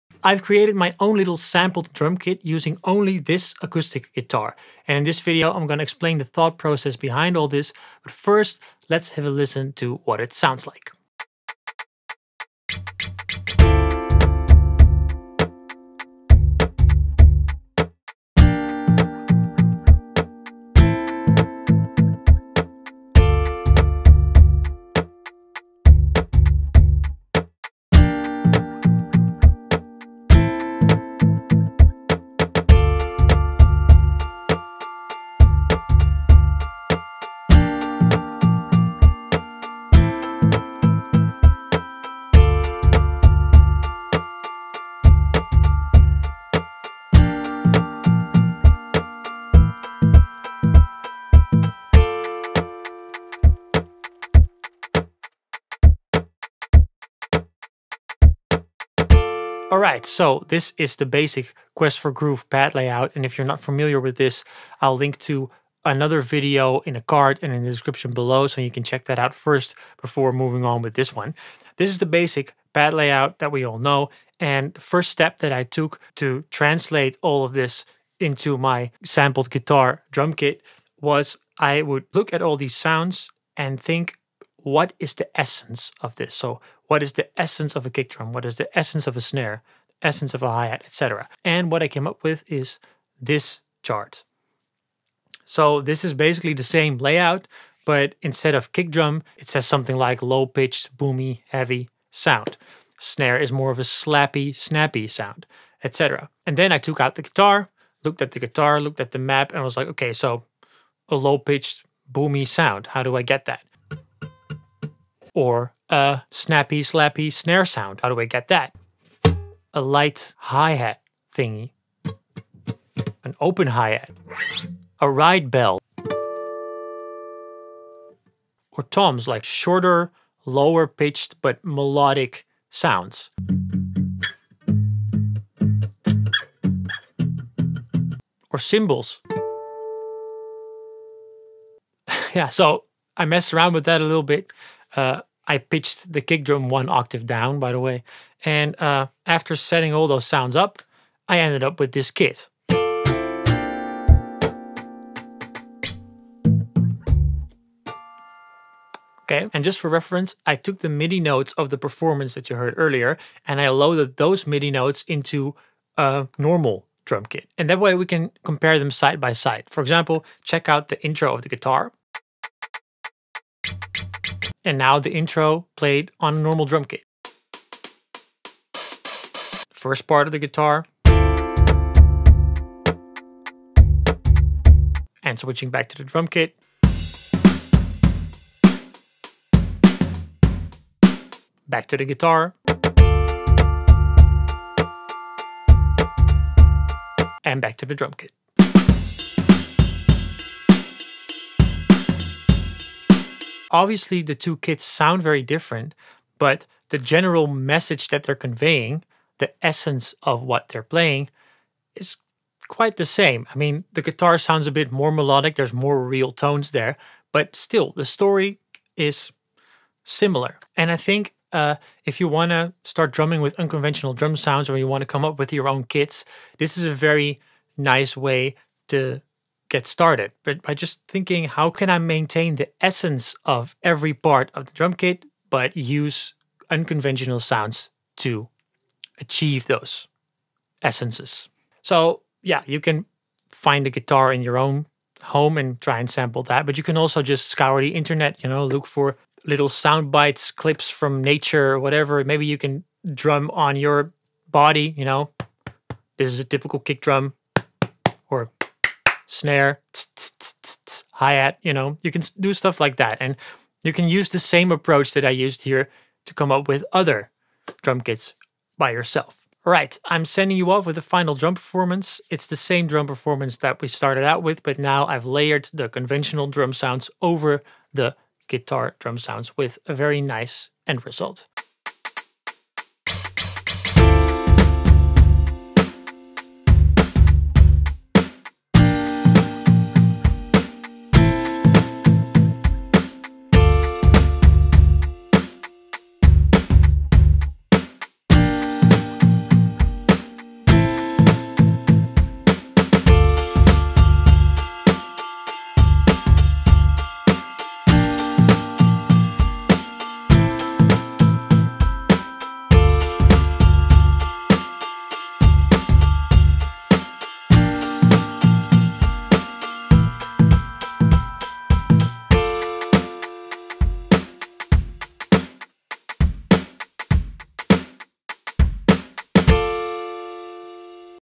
Finger drumming with an Acoustic Guitar?!?
In this video I'm gonna explain a way of thinking that you can use when setting up a finger drumming rig with unconventional sounds. It's all about figuring out the essence of what every drum sound truly is.